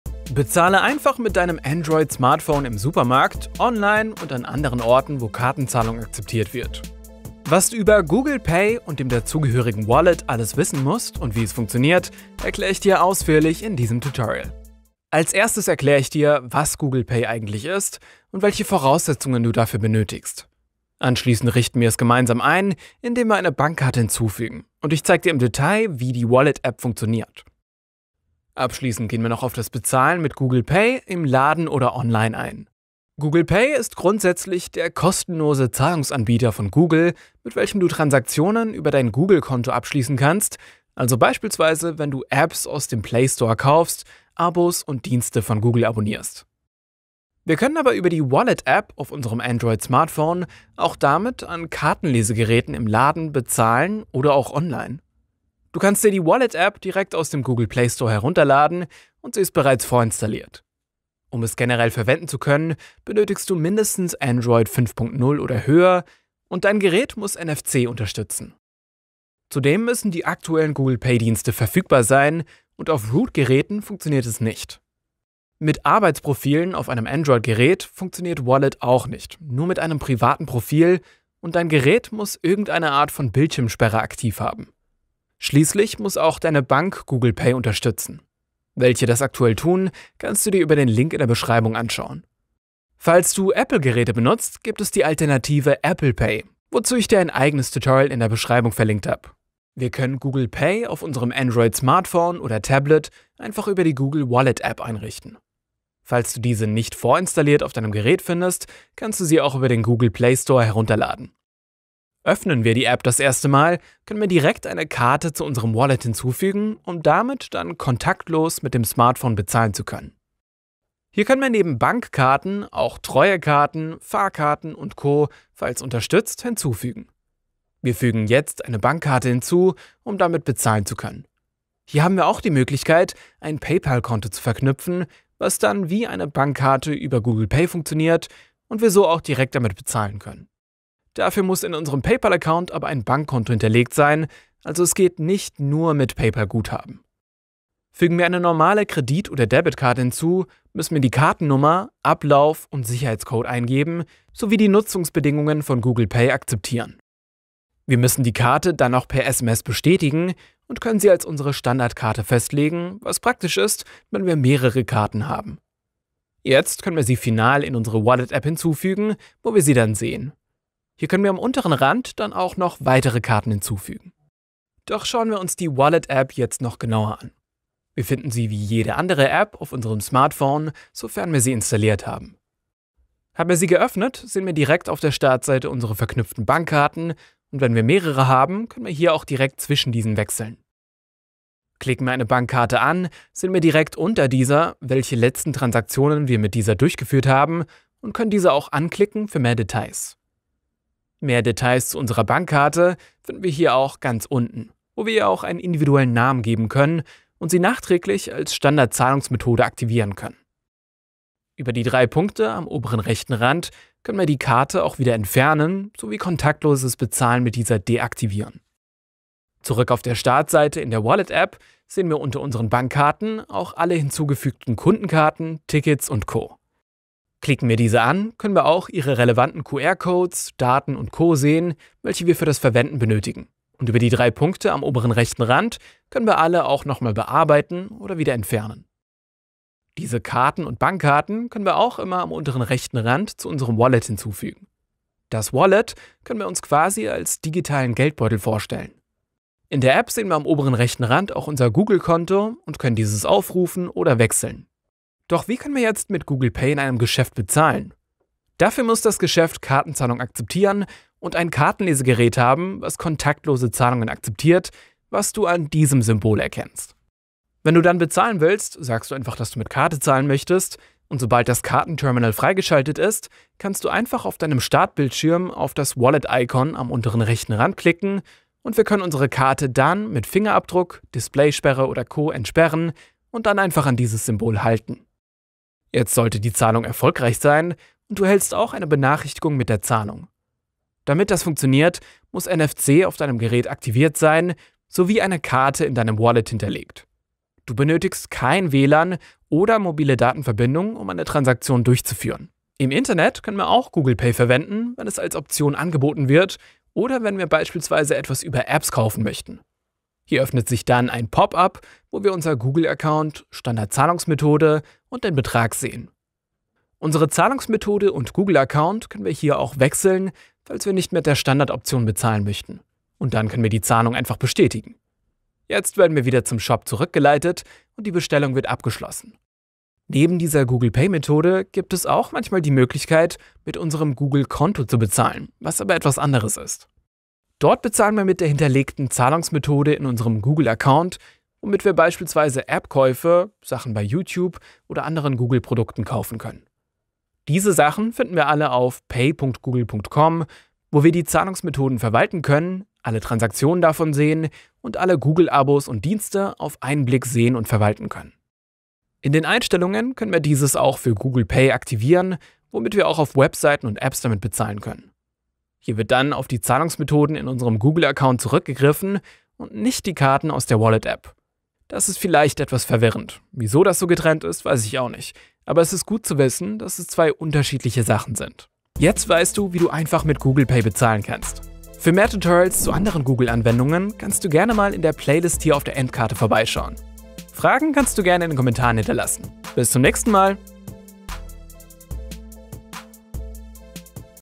Google Pay & Wallet (Tutorial): Alles was du zum kontaktlosen Bezahlen wissen musst
Was ist Google Pay?
Bezahle einfach mit deinem Android-Smartphone im Supermarkt, Online und an anderen Orten, wo Kartenzahlung akzeptiert wird. Was du über Google Pay und dem dazugehörigen Wallet alles wissen musst und wie es funktioniert, erkläre ich dir ausführlich in diesem Tutorial.

Inhaltsverzeichnis
00:00 - Einleitung
00:14 - Inhaltsverzeichnis
00:33 - Was ist Google Pay?
01:39 - Google Pay einrichten (Karte hinzufügen)
02:54 - Wallet App (Überblick): Karten verwalten
04:15 - Mit Google Pay im Laden bezahlen
05:02 - Mit Google Pay online bezahlen

Hast du noch irgendwelche Fragen? Dann stell Sie doch gerne in den Kommentaren

Produktion:
► Skript: Simon Voppmann
► Schnitt & Design: Simon Voppmann